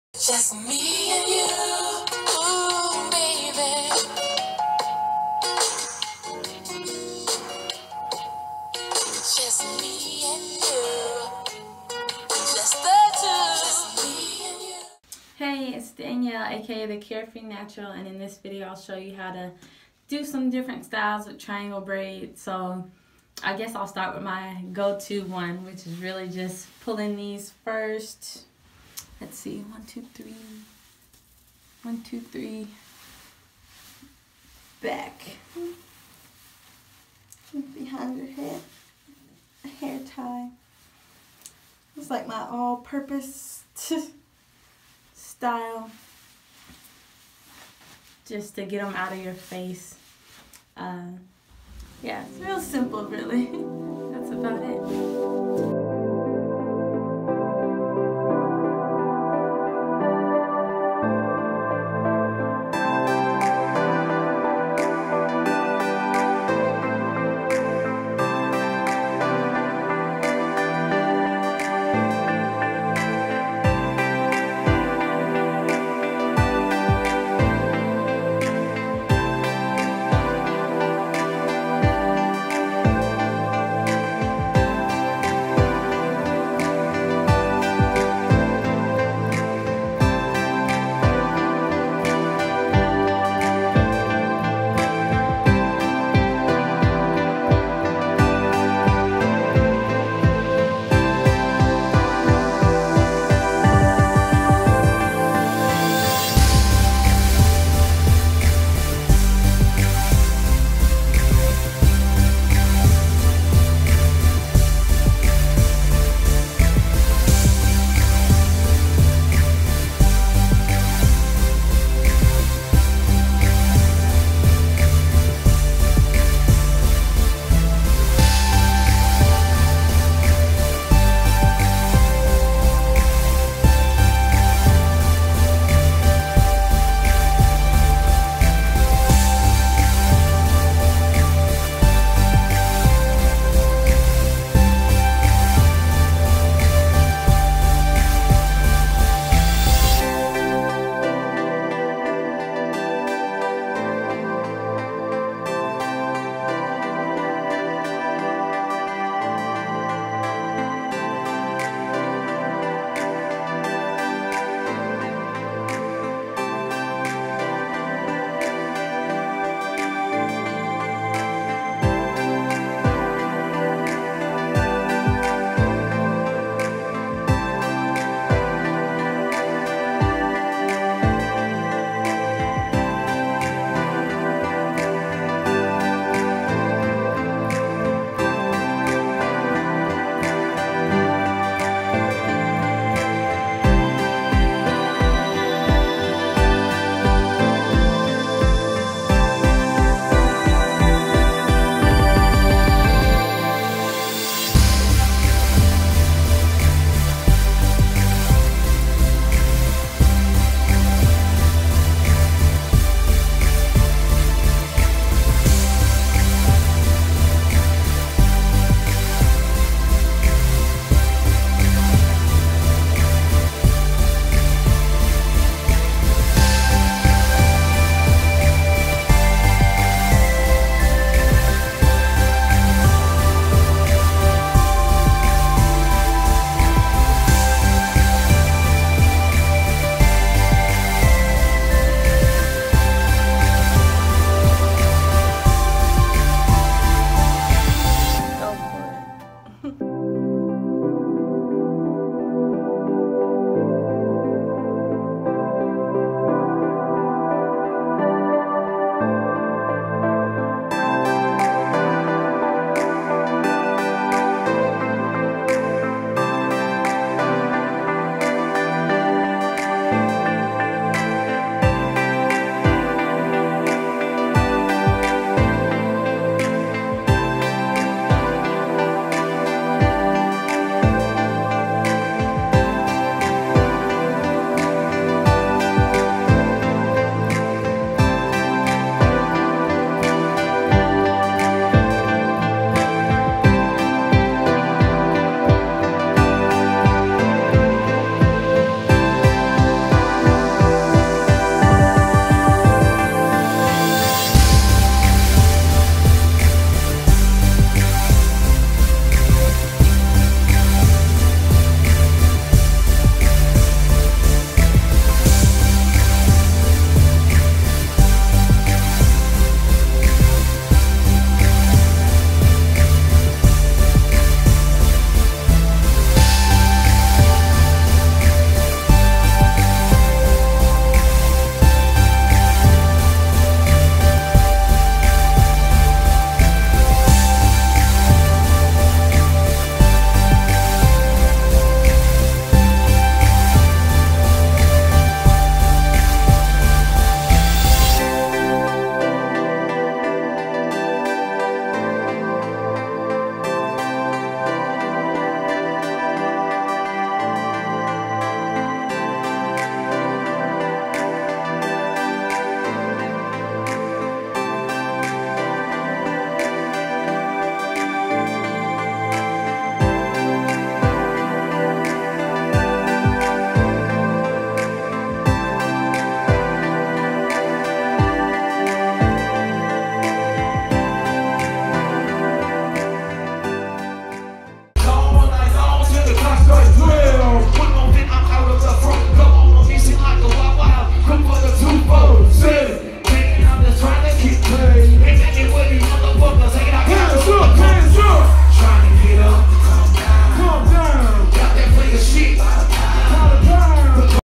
Triangle Braid Styles | Buns, Up-dos, Braided Styles and More
SC: carefreenatural
Details below

Different hair styles with triangle braids. Buns, Up-dos, Braided Styles, ponytails. I also included some "adventurous" for those who might could jazz them up and pull them off

PS I've had these braids in for about a month, so it would look a lot neater on fresh braids :P

PSS if the videos are too fast you can slow it down in the settings